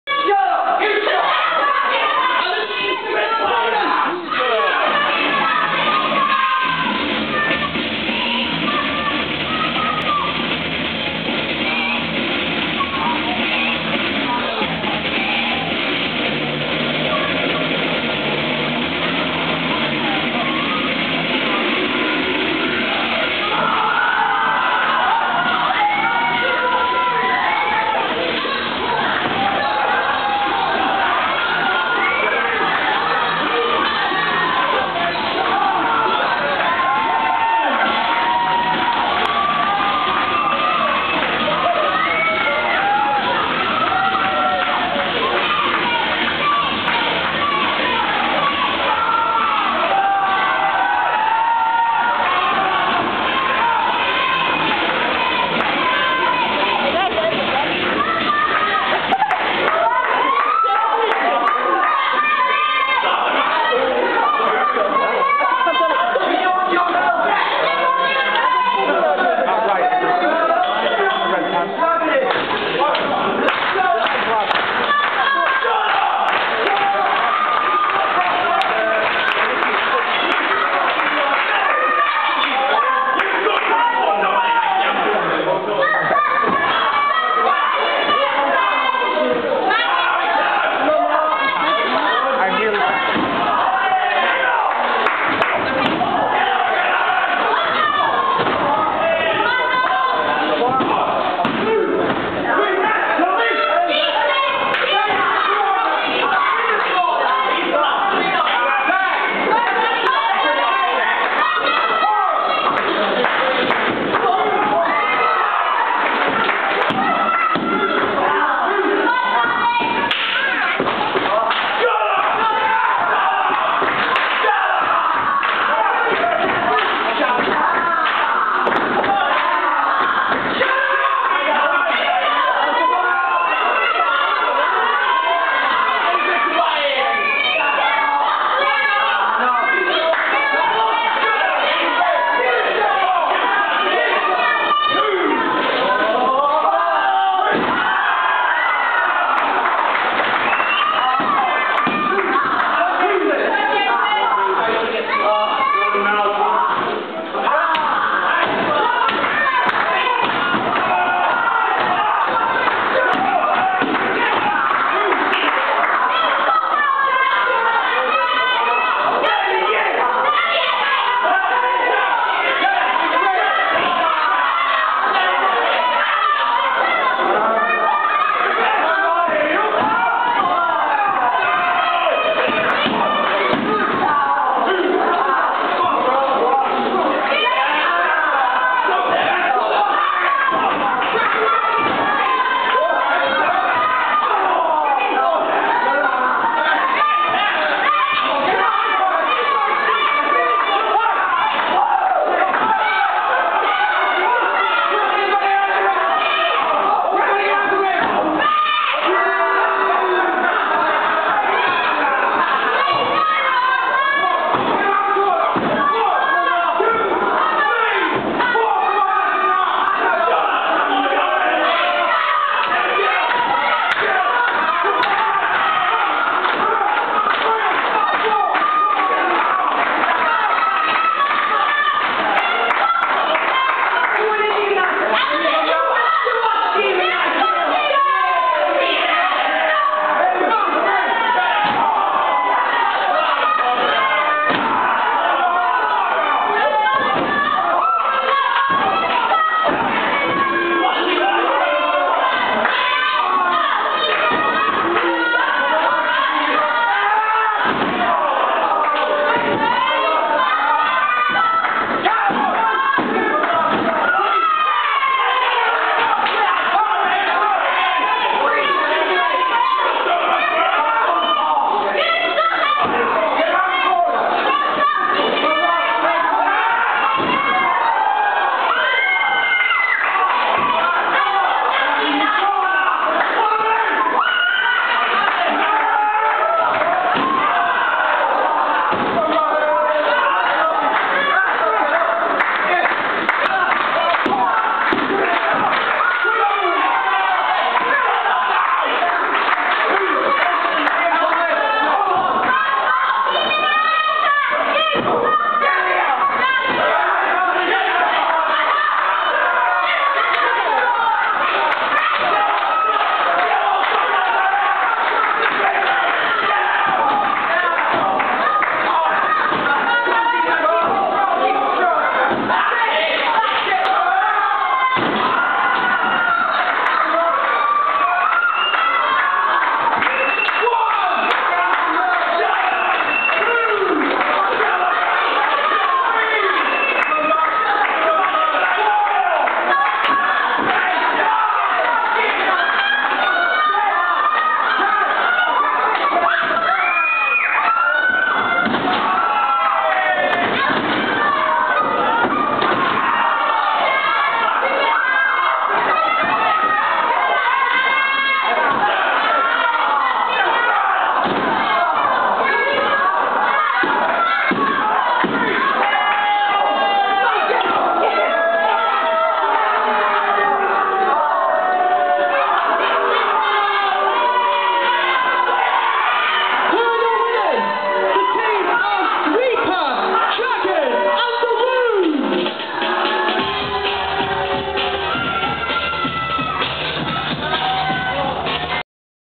RWA - 17th Oct 2009 - CKW V Respectable Robert Hearn, Danny Demento, MLW, Jason Transport
CKW Dragon, Reaper, Loons V Respectable Robert Hearn, Danny Demento, MLW, Jason Transport